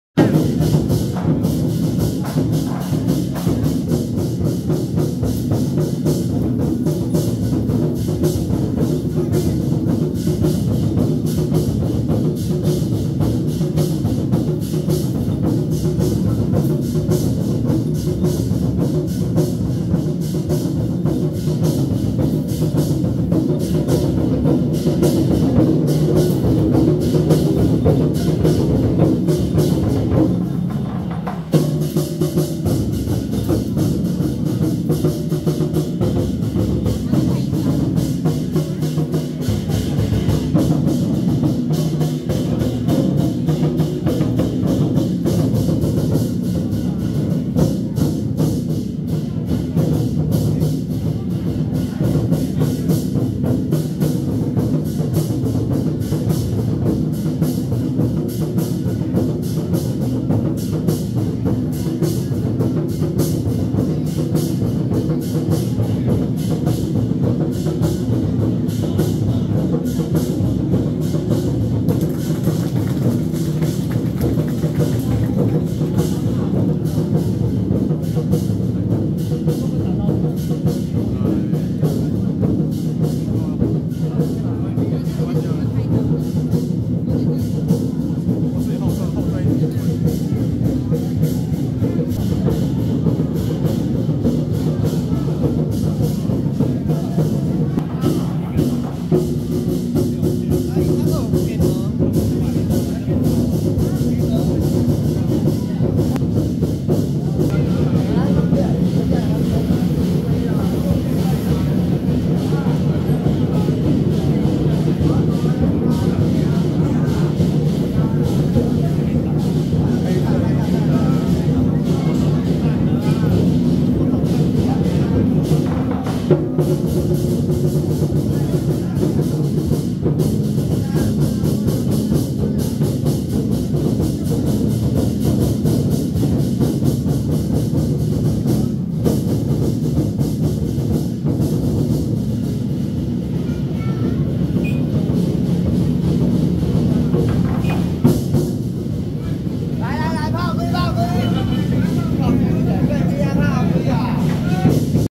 【臺北鈊泰美禮生】~113板橋聖賢堂北極玄天上帝南巡參香回駕遶境
臺北鈊泰美禮生